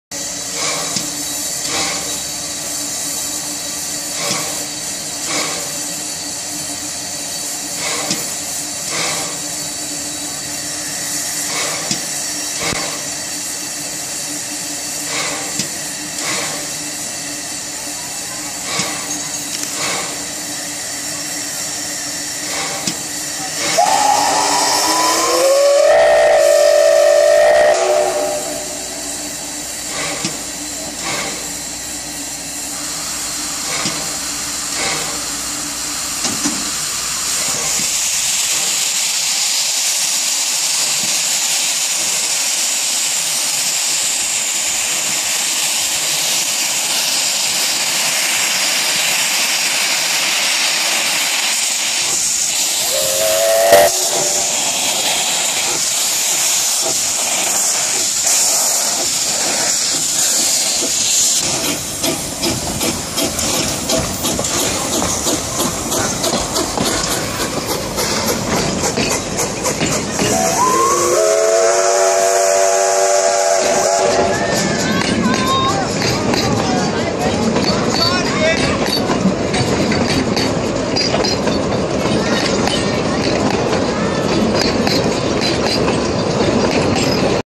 Puffing Billy at Menzies Creek
6a departs menzies creek Chas Bevan Memorial train